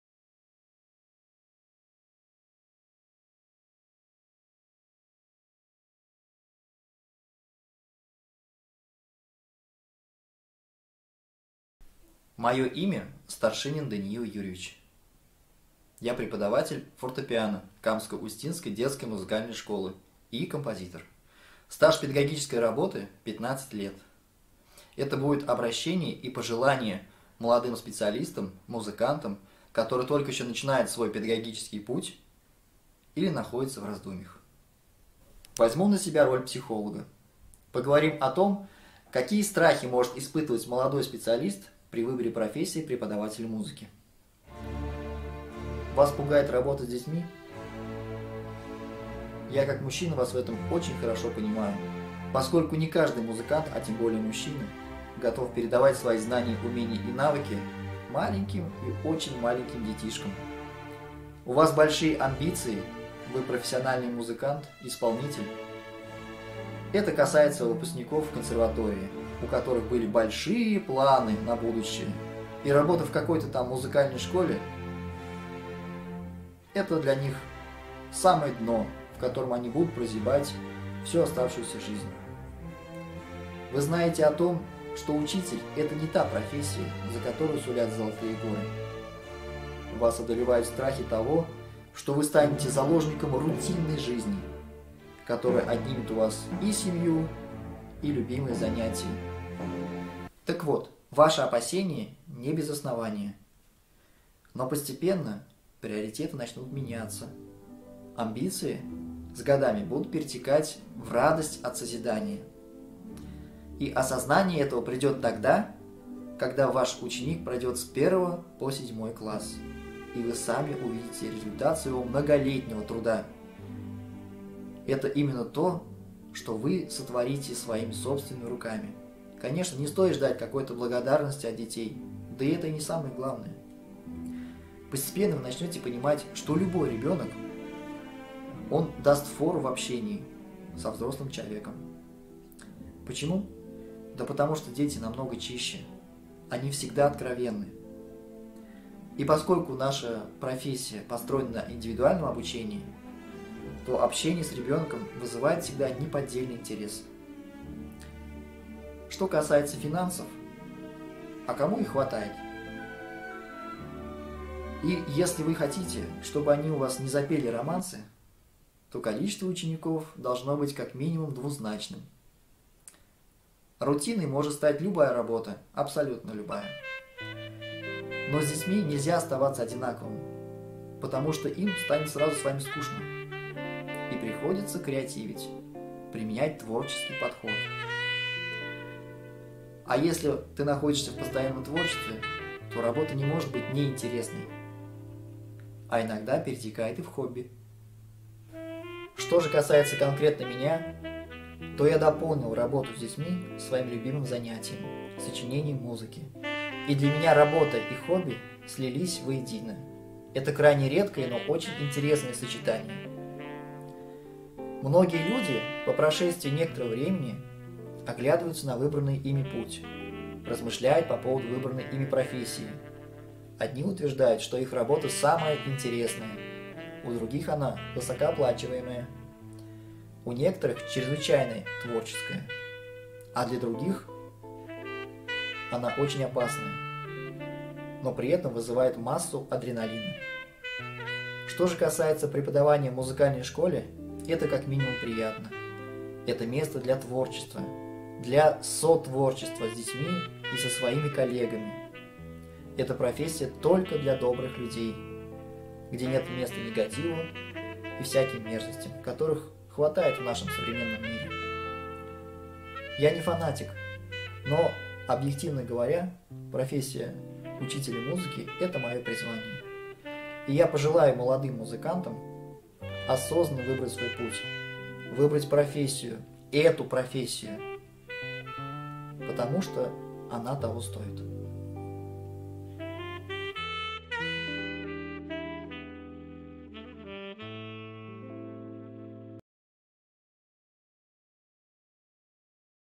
Мое педагогическое послание профессиональному сообществу.
Для 2 тура Республиканского конкурса профессионального мастерства работников сферы дополнительного образования "Сердце отдаю детям - 2021"